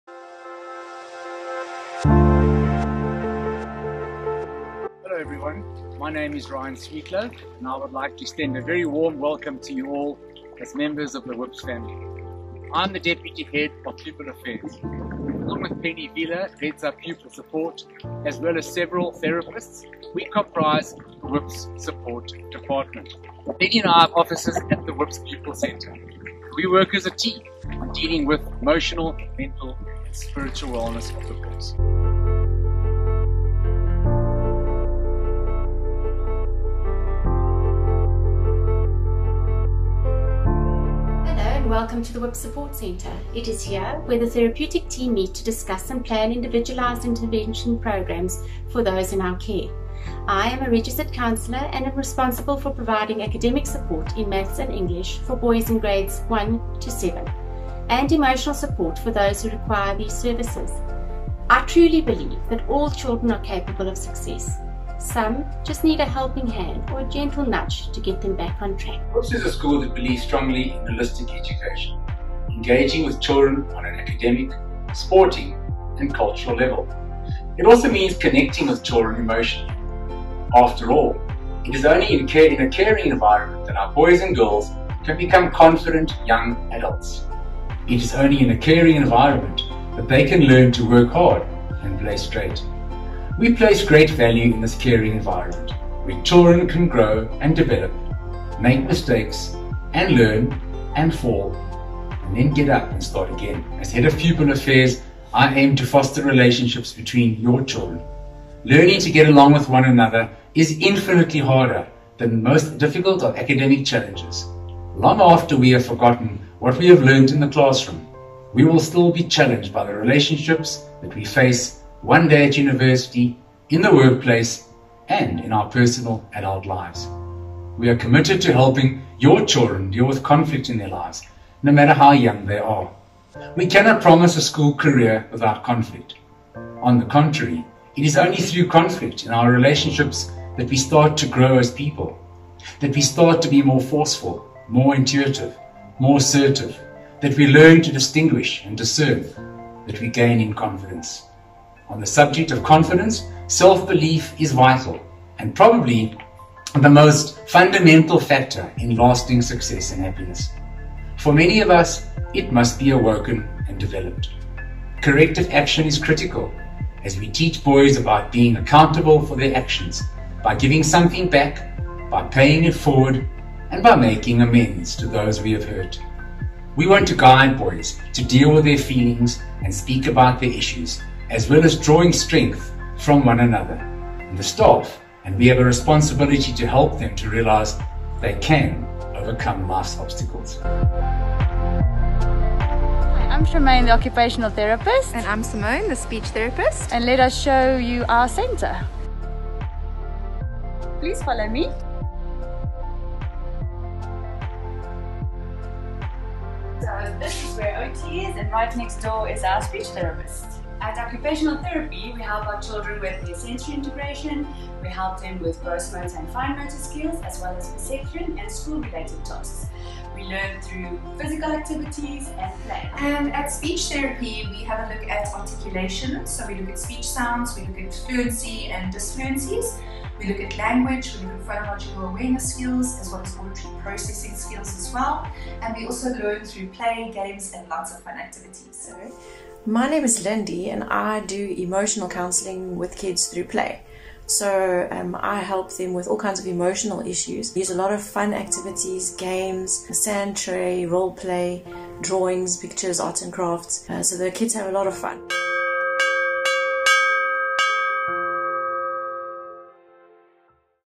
An Introduction to the WHPS Pupil Support Department
Please watch this video to meet the staff in the Pupil Support Department and find out more about what the department has to offer.